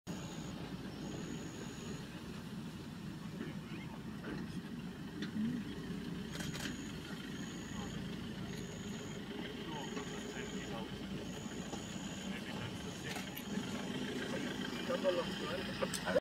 Thamba, a big male leopard strolls towards safari vehicle
This handsome, big leopard took an afternoon stroll past the safari vehicle - isn't he stunning?
Video: Marlo Hartman (Idube Guide)